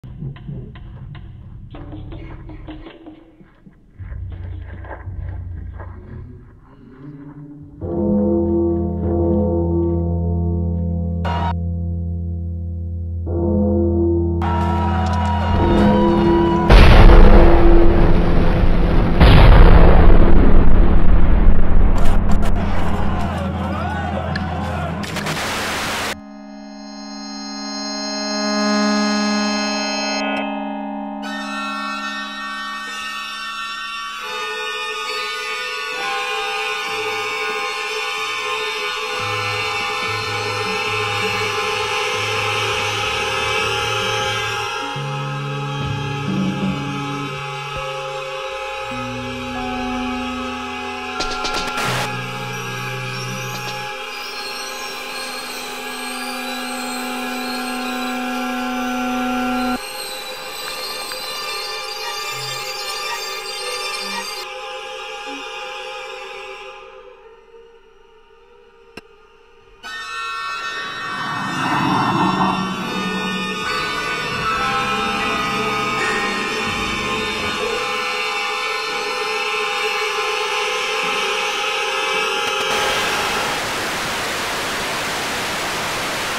Lebbeus Woods, City of Air
First semester project for visual studies class
Nathaniel Schlundt, M.Arch, University of Pennsylvania